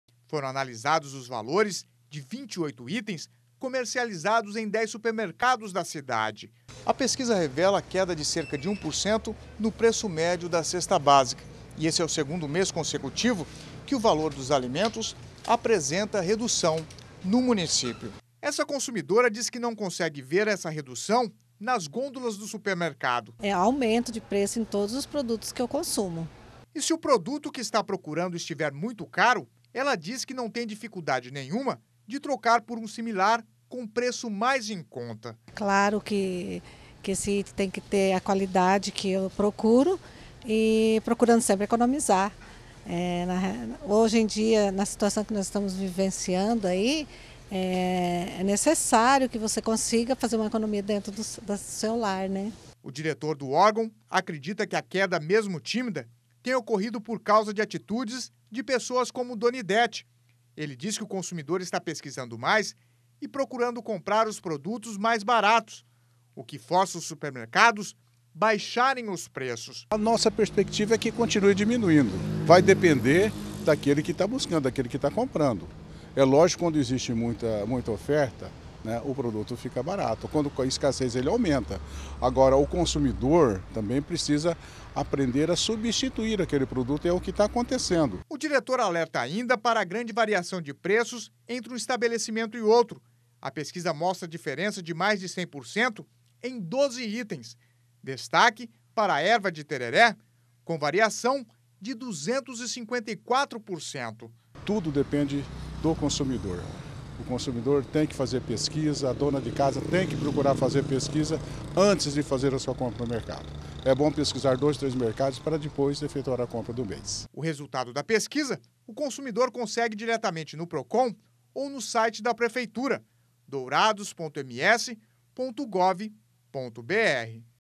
Custo da cesta básica cai pelo segundo mês seguido em Dourados
Em Dourados o preço da cesta básica também voltou a cair. Os consumidores estão pesquisando mais e comprando produtos mais em conta.